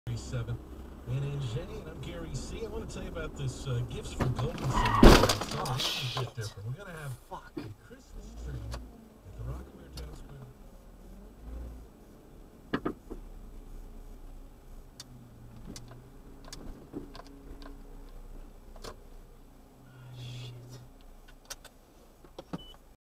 Bumper to Bumper || ViralHog
Occurred on December 6, 2016 / New Jersey, USA

"Fender bender accident. No damage to car in front other than scuff mark. Driver's car sustained minor damage."